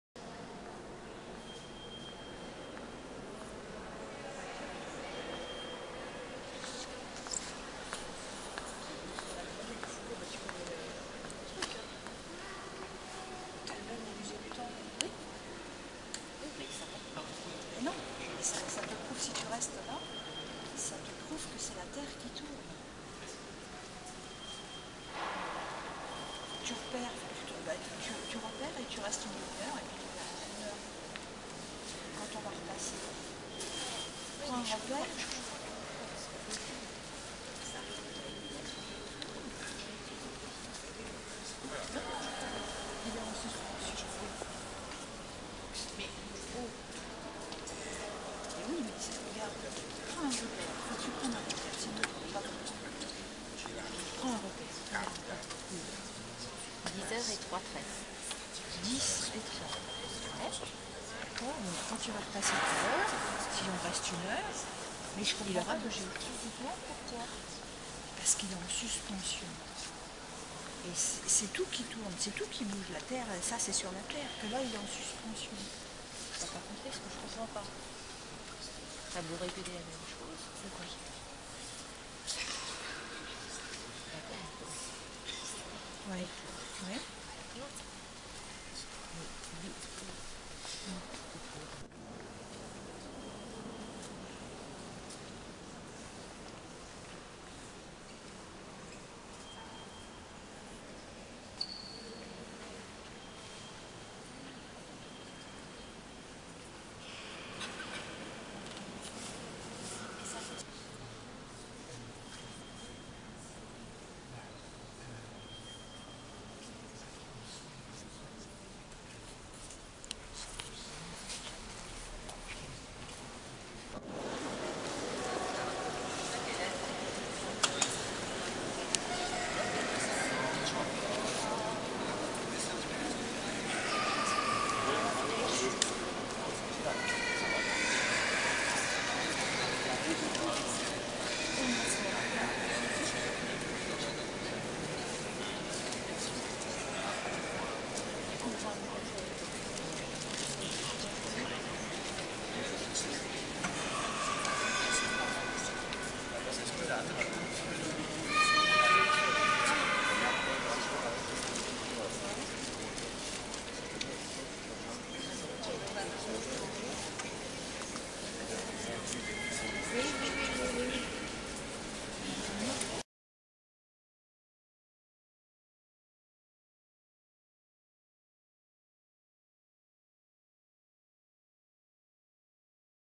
Projet Horloges d'Altitude : Le pendule de Foucault au Panthéon.
Cette vidéo présente le pendule de Foucault au Panthéon à 10 heures et à 11 heures. Le pendule de Foucault, du nom du physicien français Léon Foucault, est un dispositif expérimental conçu pour mettre en évidence la rotation de la Terre par rapport à un référentiel galiléen. Pas si simple à mettre en œuvre et à comprendre, mais reposant à observer. Merci à tous les acteurs de ce projet. A suivre…